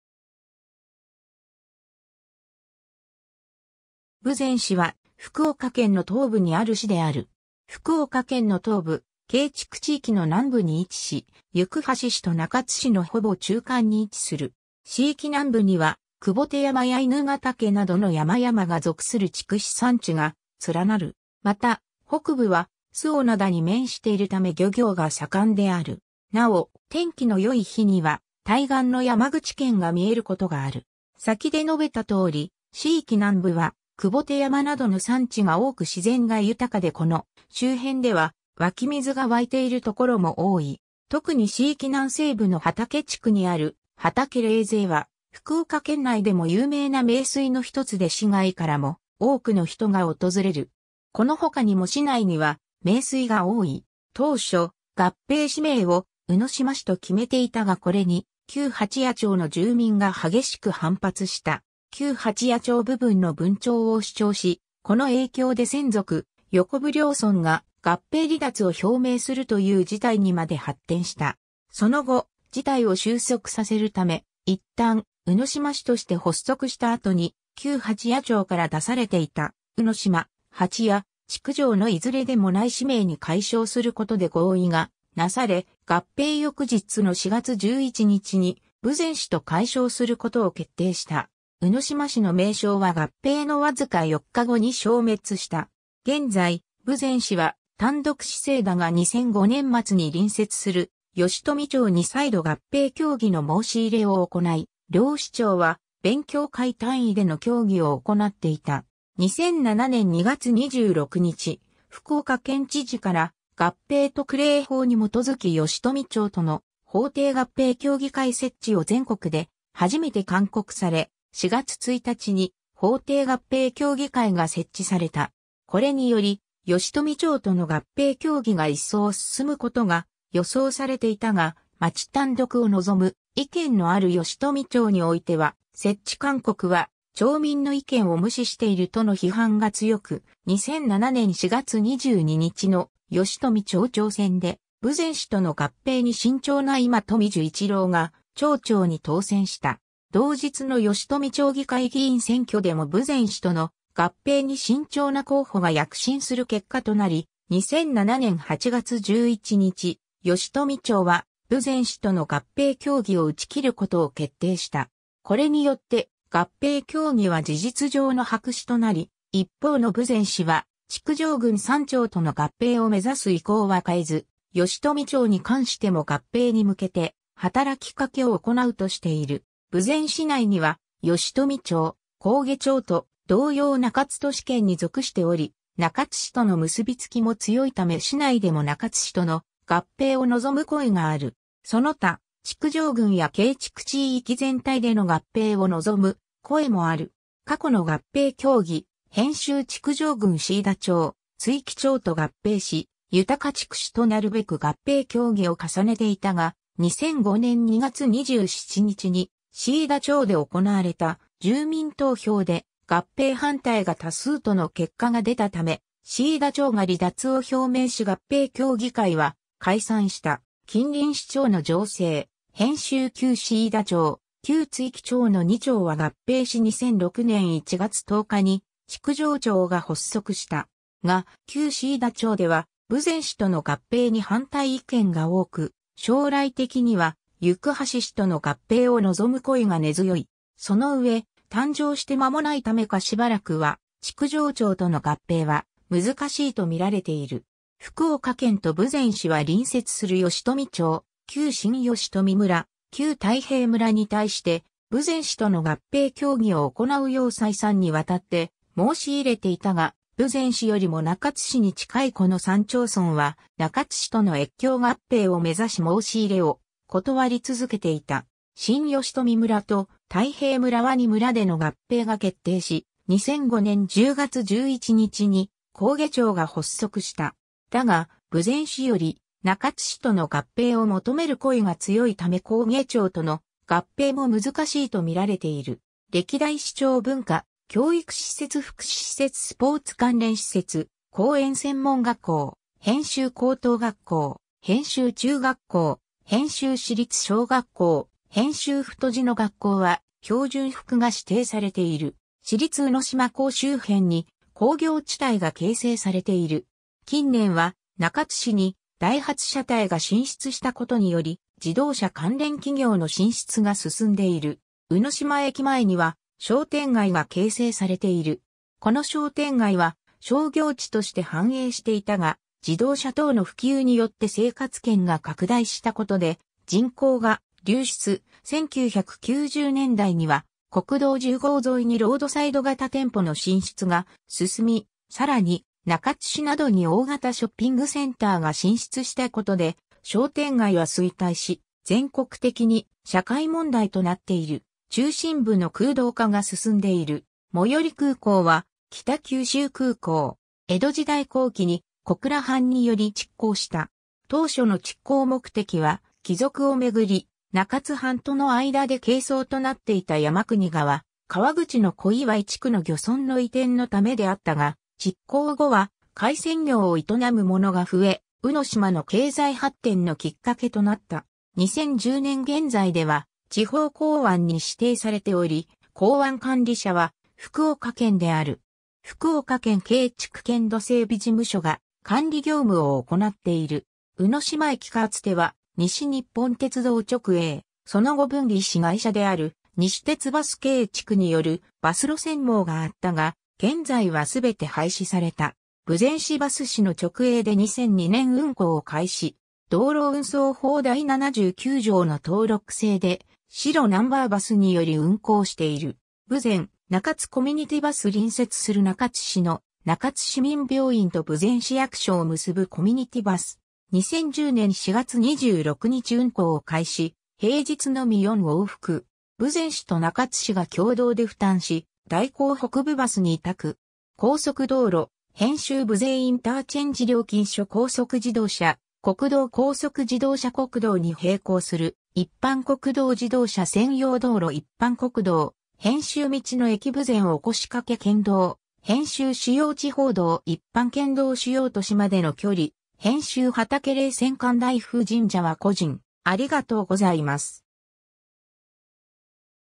豊前市
豊前市（ぶぜんし）は、福岡県の東部にある市である。
 福岡県の東部、京築地域の南部に位置し行橋市と中津市のほぼ中間に位置する。
市域南部には求菩提山や犬ヶ岳などの山々が属する筑紫山地が連なる。
また、北部は周防灘（瀬戸内海）に面しているため漁業が盛んである。
なお、天気の良い日には対岸の山口県が見えることがある。
 先で述べたとおり、市域南部は求菩提山などの山地が多く自然が豊かでこの周辺では湧き水が湧いている所も多い。
特に市域南西部の畑地区にある「畑冷泉（はたのれいせん）」は、福岡県内でも有名な名水の一つで市外からも多くの人が訪れる。
この他にも市内には名水が多い。
 ※当初、合併市名を宇島市と決めていたがこれに旧八屋町の住民が激しく反発した。
旧八屋町部分の分町を主張し、この影響で千束・横武両村が合併離脱を表明するという事態にまで発展した。
その後、事態を収束させるため、一旦宇島市として発足した後に旧八屋町から出されていた“宇島”・“八屋”・“築上”のいずれでもない市名に改称することで合意がなされ、合併翌日の4月11日に豊前市と改称することを決定した。
宇島市の名称は合併の僅か4日後に消滅した。
 現在、豊前市は単独市制だが2005年末に隣接する吉富町に再度合併協議の申し入れを行い、両市町は勉強会単位での協議を行っていた。
2007年2月26日、福岡県知事から、合併特例法に基づき吉富町との法定合併協議会設置を全国で初めて勧告され、4月1日に法定合併協議会が設置された。
これにより、吉富町との合併協議が一層進むことが予想されていたが、町単独を望む意見のある吉富町においては「設置勧告は町民の意見を無視している」との批判が強く、2007年4月22日の吉富町長選で、豊前市との合併に慎重な今冨寿一郎が町長に当選した。
同日の吉富町議会議員選挙でも豊前市との合併に慎重な候補が躍進する結果となり、2007年8月11日、吉富町は豊前市との合併協議を打ち切ることを決定した。
これによって合併協議は事実上の白紙となり、一方の豊前市は築上郡3町との合併を目指す意向は変えず、吉富町に関しても合併に向けて働きかけを行うとしている。
 豊前市内には吉富町、上毛町（旧新吉富村、旧大平村）と同様中津都市圏に属しており、中津市との結びつきも強いため市内でも中津市との合併を望む声がある。
その他、築上郡や京築地域全体での合併を望む声もある。
 過去の合併協議[編集] 築上郡椎田町、築城町と合併し「豊築市（ほうちくし）」となるべく合併協議を重ねていたが、2005年（平成17年）2月27日に椎田町で行われた住民投票で合併反対が多数との結果が出たため、椎田町が離脱を表明し合併協議会は解散した。
 近隣市町の情勢[編集] 旧椎田町、旧築城町の2町は合併し2006年1月10日に「築上町」が発足した。
が、旧椎田町では豊前市との合併に反対意見が多く、将来的には行橋市との合併を望む声が根強い。
その上、誕生して間もないためかしばらくは築上町との合併は難しいと見られている。
 福岡県と豊前市は隣接する吉富町、旧新吉富村、旧大平村に対して、豊前市との合併協議を行うよう再三にわたって申し入れていたが、豊前市よりも中津市に近いこの3町村は、中津市との越境合併を目指し申し入れを断り続けていた。
新吉富村と大平村は2村での合併が決定し、2005年10月11日に上毛町が発足した。
だが、豊前市より中津市との合併を求める声が強いため上毛町との合併も難しいと見られている。
 ※歴代市長 文化・教育施設 福祉施設 スポーツ関連施設 公園 専門学校[編集] 高等学校[編集] 中学校[編集] 市立 小学校[編集] 太字の学校は標準服が指定されている。
 市立 宇島港周辺に工業地帯が形成されている。
近年は中津市にダイハツ車体が進出した事により、自動車関連企業の進出が進んでいる。
 宇島駅前には商店街が形成されている。
この商店街は商業地として繁栄していたが、自動車等の普及によって生活圏が拡大したことで人口が流出、1990年代には国道10号沿いにロードサイド型店舗の進出が進み、さらに中津市などに大型ショッピングセンターが進出したことで商店街は衰退し、全国的に社会問題となっている中心部の空洞化が進んでいる。
 最寄り空港は北九州空港（北九州市・苅田町）。
 江戸時代後期に小倉藩により築港した。
当初の築港目的は、帰属を巡り中津藩との間で係争となっていた山国川河口の小祝地区（現在は中津市内）の漁村の移転のためであったが築港...